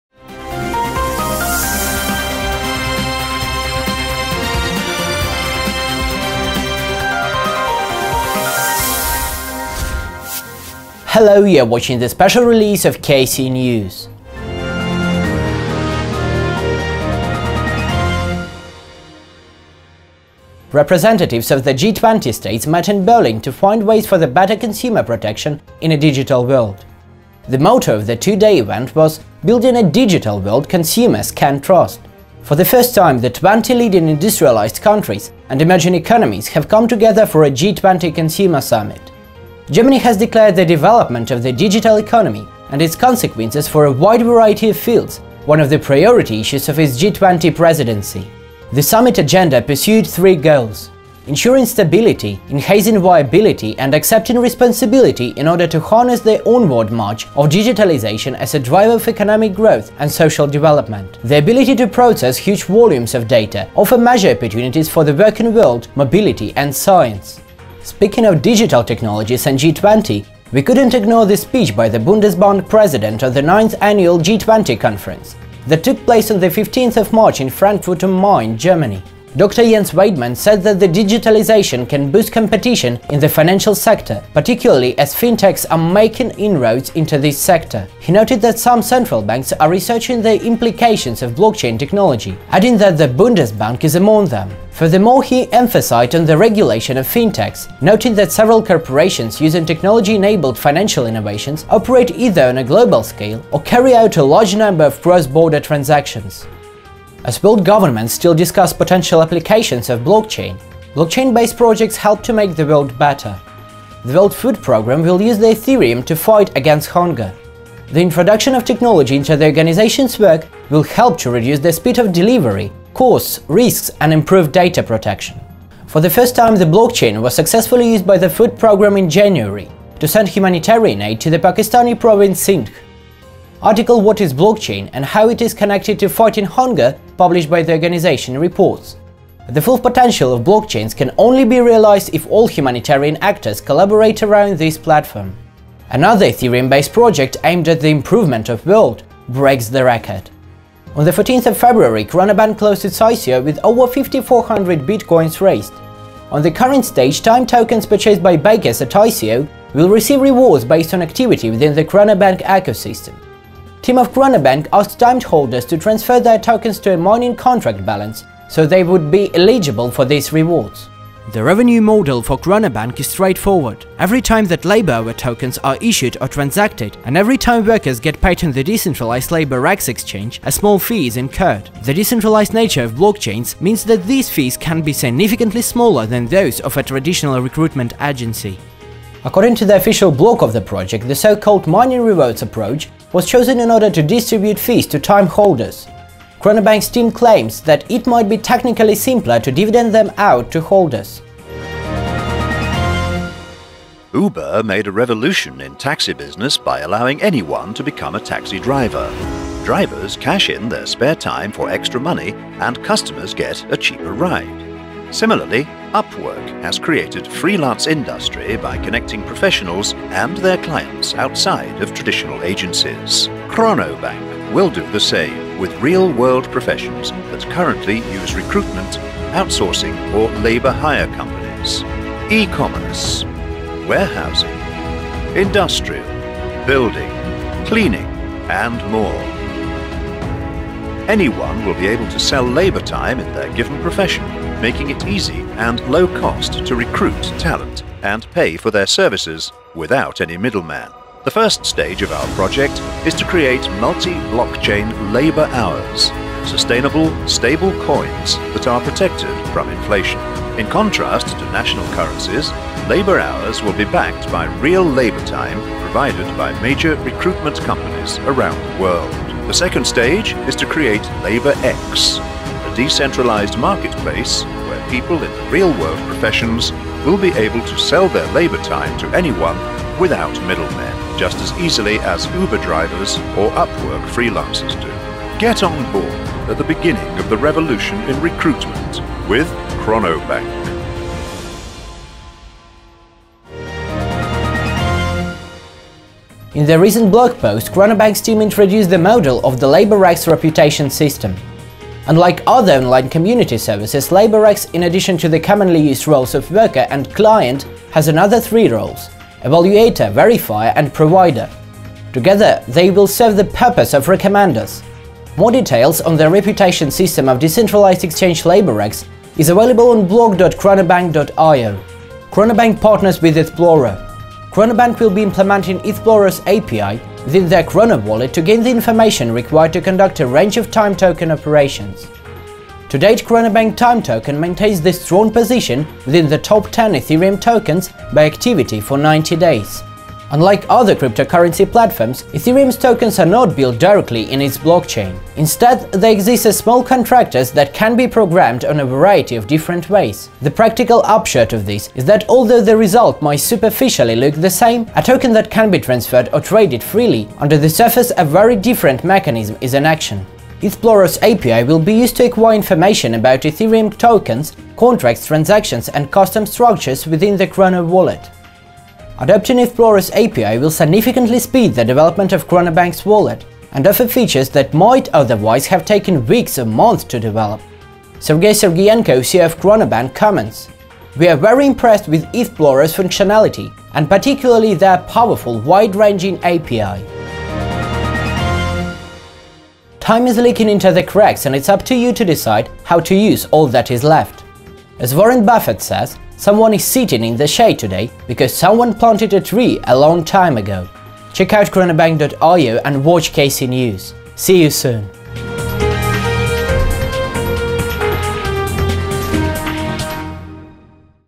KCN Special release. Chronobank #8
Representatives of the G20 states met in Berlin to find ways for the better consumer protection in a digital world. The motto of the two-day event was "Building a digital world consumers can trust".

For the first time, the 20 leading industrialised countries and emerging economies have come together for a G20 consumer summit. Germany has declared the development of the digital economy and its consequences for a wide variety of fields one of the priority issues of its G20 Presidency.

The summit agenda pursued three goals: ensuring stability, enhancing viability and accepting responsibility in order to harness the onward march of digitalisation as a driver of economic growth and social development. The ability to process huge volumes of data offers major opportunities for the working world, mobility and science.

Speaking of digital technologies and G20, we couldn’t ignore the speech by the Bundesbank president on the 9th Annual G20 Conference, that took place on the 15th of March in Frankfurt am Main, Germany.
Dr Jens Weidmann said that digitization can boost competition in the financial sector, particularly as fintechs are making inroads into this sector. He noted that some central banks are researching the implications of blockchain technology, adding that the Bundesbank is among them. Furthermore, he emphasized on the regulation of fintechs, noting that several corporations using technology-enabled financial innovations operate either on a global scale or carry out a large number of cross-border transactions.

As world governments still discuss potential application of blockchain, Blockchain- based projects help to make the world better. The World Food Program will use the ethereum to fight against hunger. The introduction of technology into the organization's work will help to reduce the speed of delivery, costs, risks and improve data protection.
For the first time the blockchain was successfully used by the food program in January, to send humanitarian aid to the Pakistani province Sindh. Article "What is 'Blockchain' and How is it connected to Fighting Hunger?", published by the organization, reports: "The full potential of blockchains can only be realized if all humanitarian actors collaborate around this platform."

Another ethereum-based project, aimed at the improvement of world, breaks the record.

On the fourteenth of February ChronoBank closed it’s ICO with over 54 hundred BTC raised. On the current stage, TIME tokens purchased by backers at ICO will receive rewards based on activity within the ChronoBank ecosystem. Team of ChronoBank asks TIME holders to transfer their tokens to a mining contract balance, so they would be eligible for these rewards.

The revenue model for ChronoBank is straightforward: every time that Labour Hour (LH) tokens are issued or transacted, and every time workers get paid on the decentralised LaborX exchange, a small fee is incurred. The decentralised nature of the blockchain means that these fees can be significantly smaller than those of a traditional recruitment agency.
According to the official blog of the project, the so-called “mining rewards“ approach was chosen in order to distribute fees to TIME holders. ChronoBank’s team claims, that it might be technically simpler to dividend them out to holders.